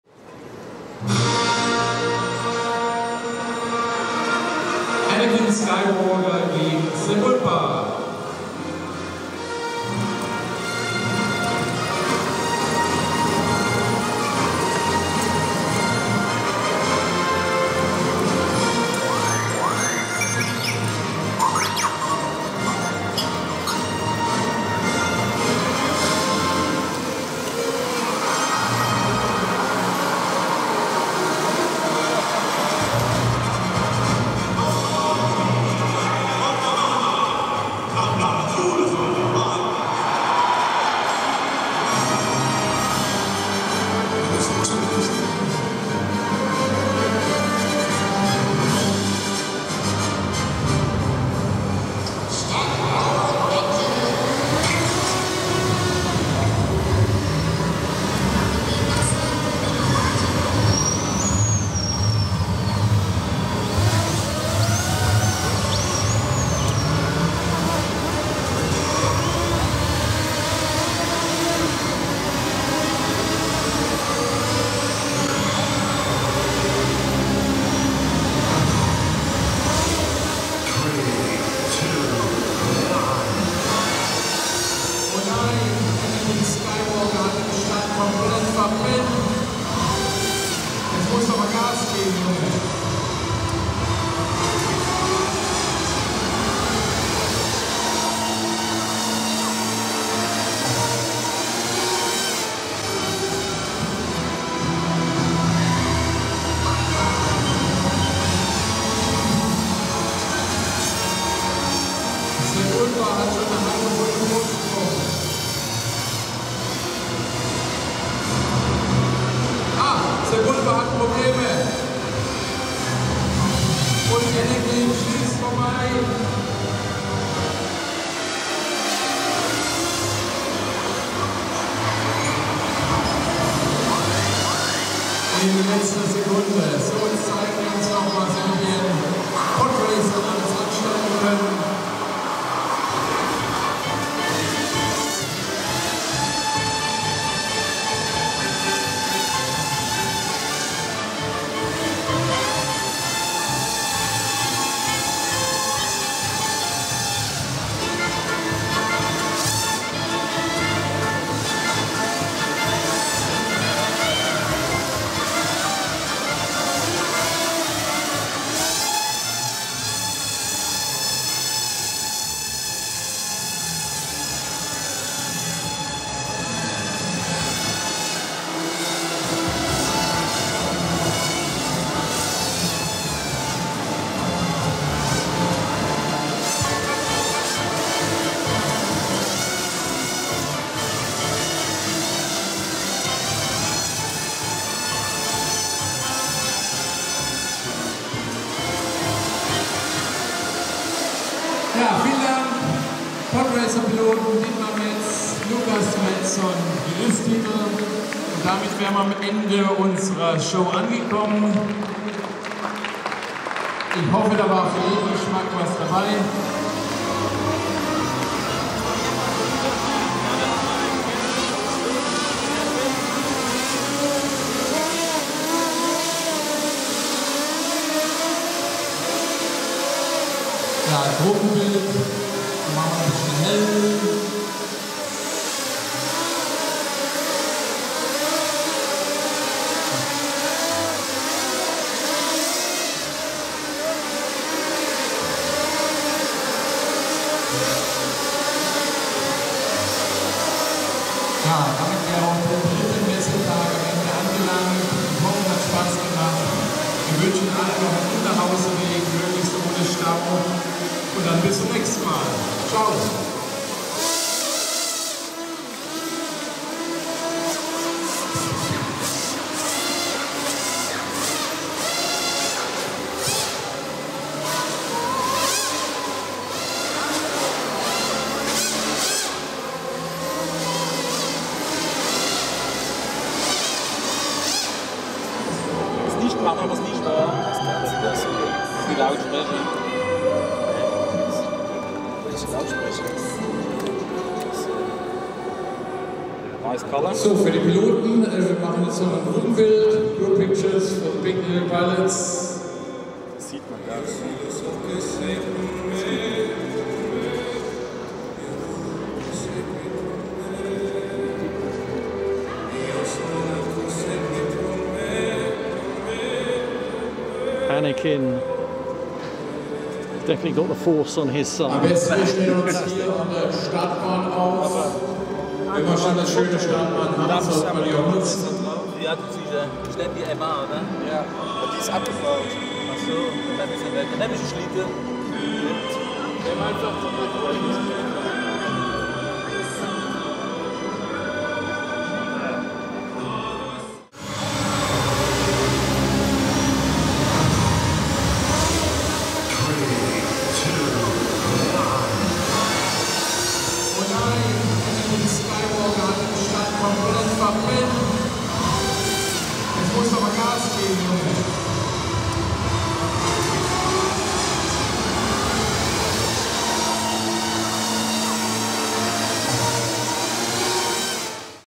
STAR WARS radio controlled (RC) Pod Racer ACTION ! (The Phantom Menace POD RACING)
The RC innovation of the DMFV Indoor Team, amazes the crowd at the Modell-Hobby-Spiel Trade Fair in Leipzig, Germany. The music started playing and the excitement and anticipation grew...then pod racers drove out to the start line along the floor and then started their engines and hovered ready to start. The race began and they were off, but as in the film, Anakin was left on the start line with engine problems with the crafty, vicious Dug, Sebulba, streaking off into the lead. But who won the race ?

Dietmar designed and constructed these models. He flies his Pod Racer together with his son Lukas and friend Chris. Just outstanding !

Why not try to make your own light weight models from Depron.

Filmed by Dom Mitchell at Leipzig Modell-Hobby-Spiel Trade Fair for the 'Essential RC' YouTube channel using the Panasonic AG-UX180 Pro 4K camcorder.